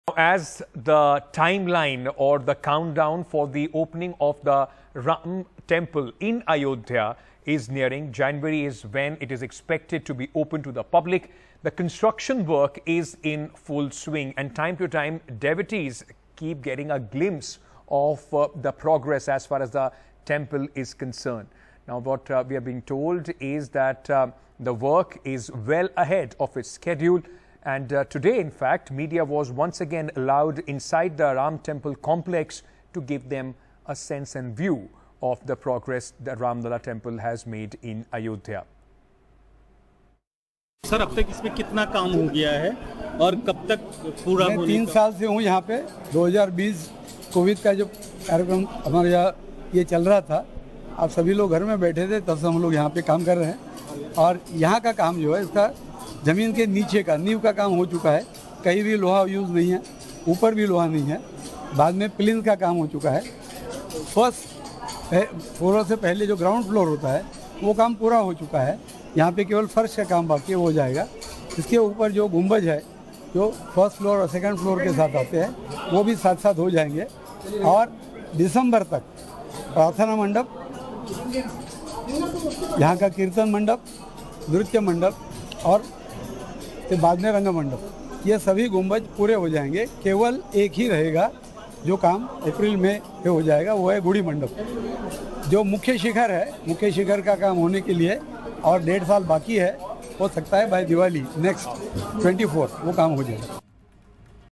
Ayodhya Ram Temple Construction In Full Swing
Ayodhya Temple construction work in full swing, devotees will soon be able to see Lord Ram.